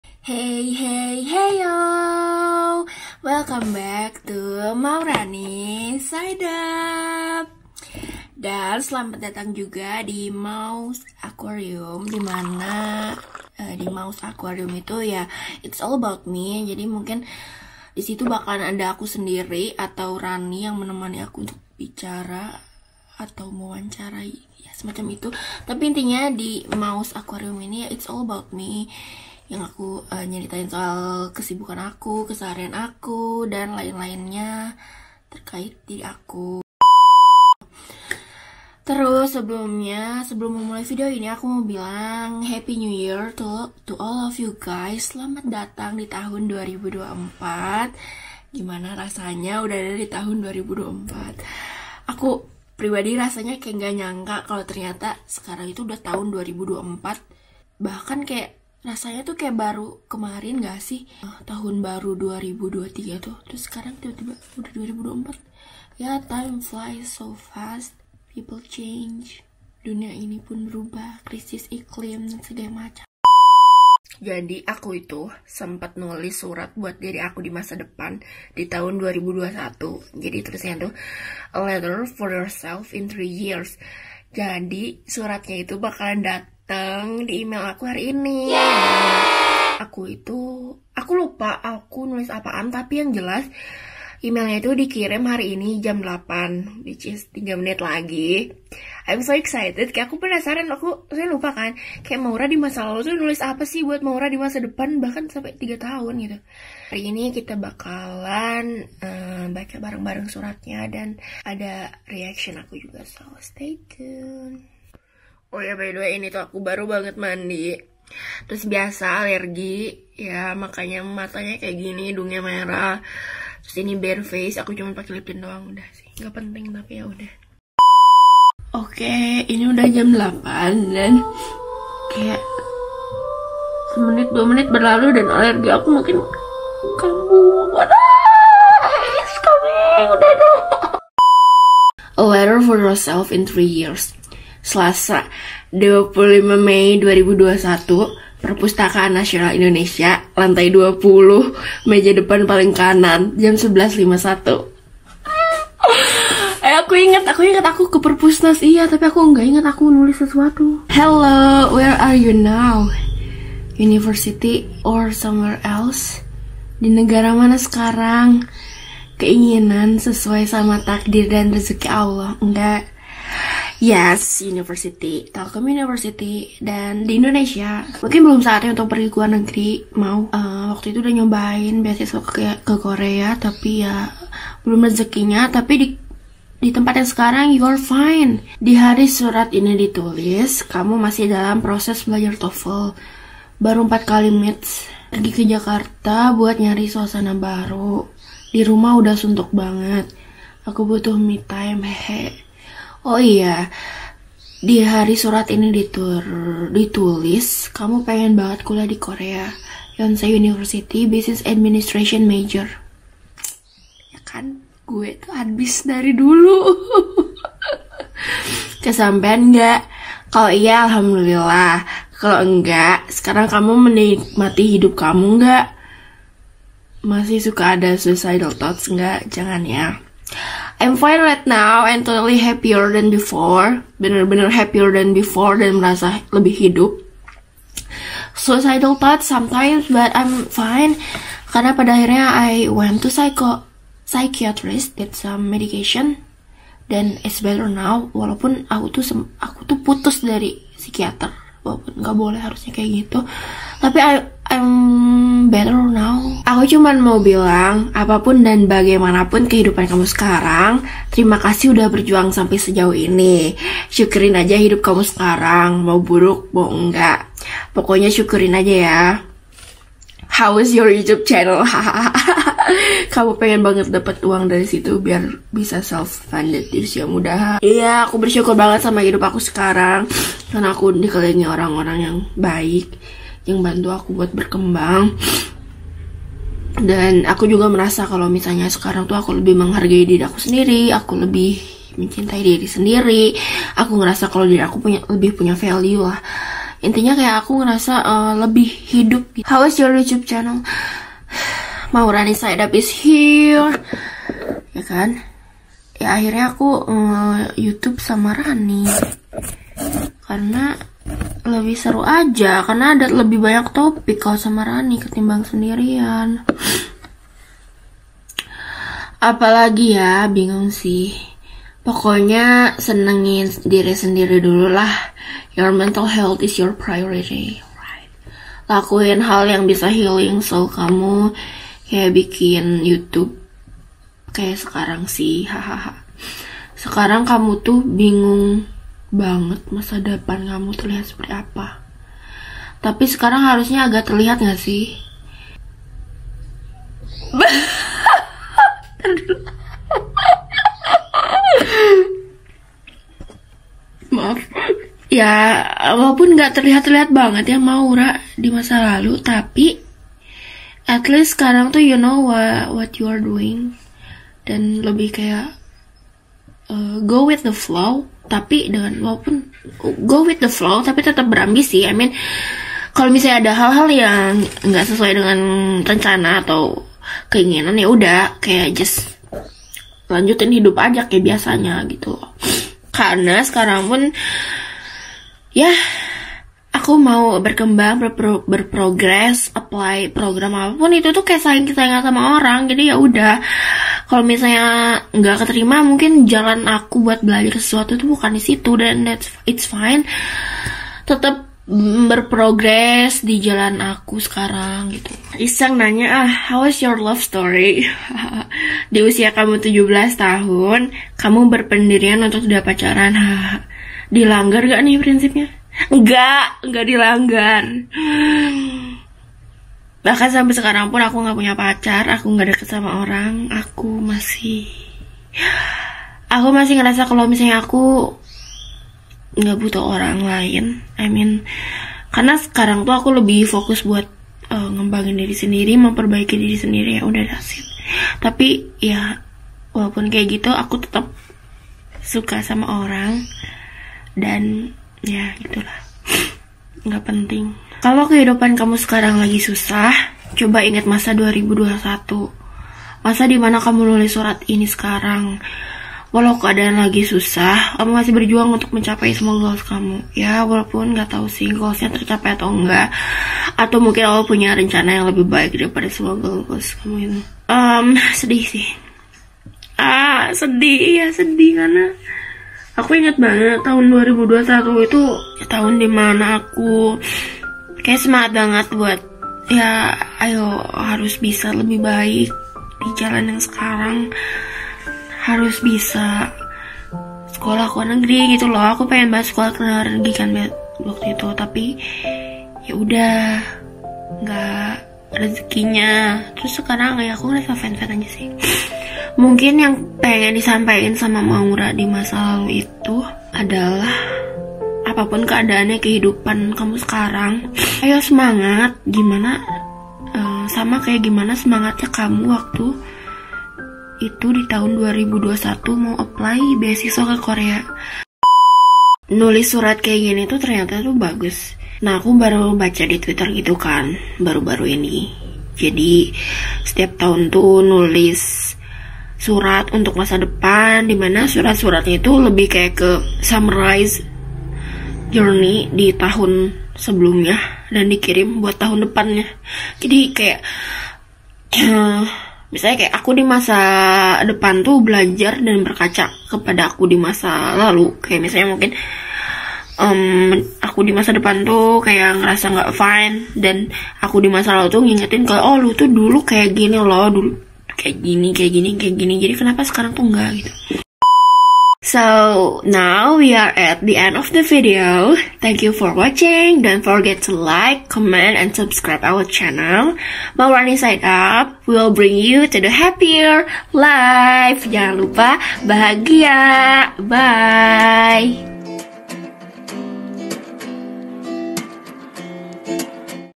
BACA SURAT DARI MASA LALU 💌🙀 | Maw's Aquarium
Hey hey heyyoowww!
Welcome back to MawRanny Side Up~

Ayooo kita bareng-bareng baca surat yang Maw tulis di tahun 2021!

Side Up!